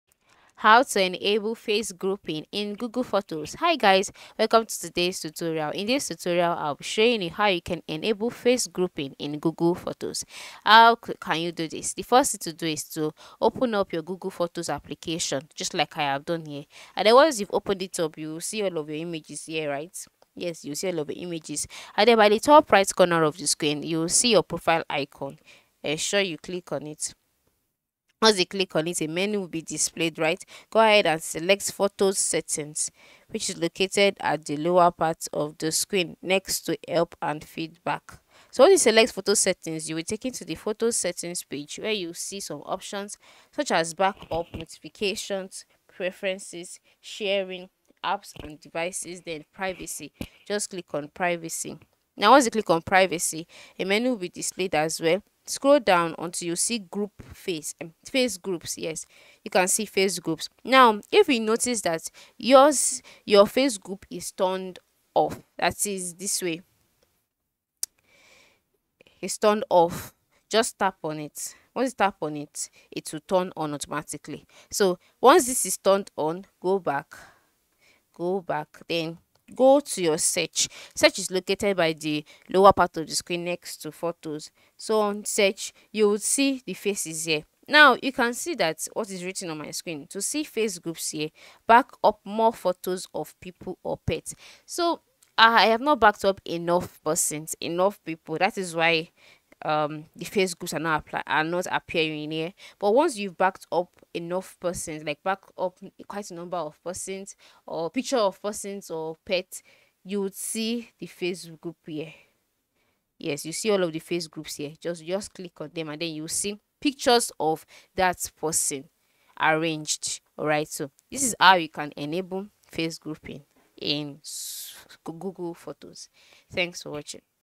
How To Enable Face Grouping in Google Photos (Quick Tutorial)
Today we talk about How To Enable Face Grouping in Google Photos, so stay until the end of the video to see the full explanation.